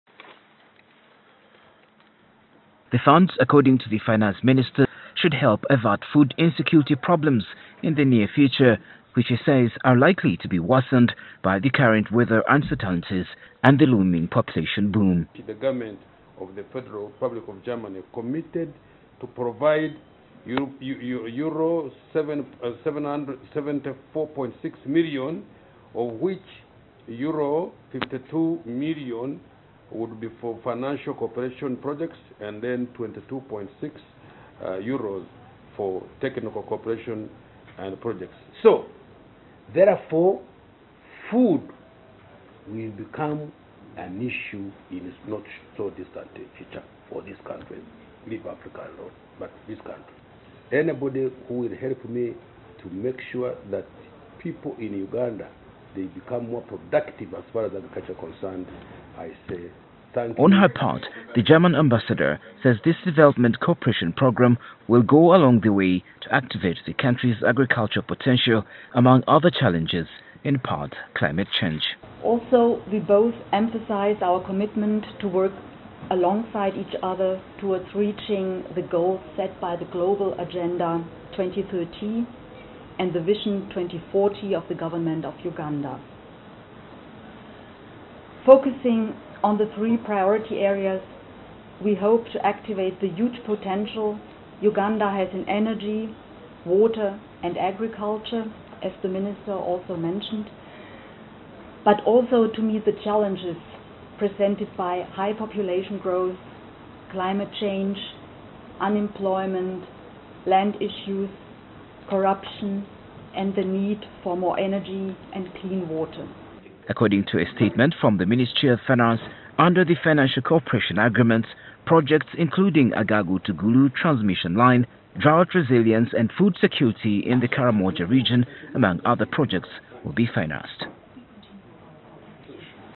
Germany gives Uganda 100M Euros for developing water and agriculture sectors
Finance minister Matia Kasaija has today signed a partnership agreement with the German Government in which Uganda will receive 100 million Euros to support the areas off water, Energy and Agriculture. According to Kasaija, intervention in the area of agricultural productivity are taking on more than usual priority in Uganda today.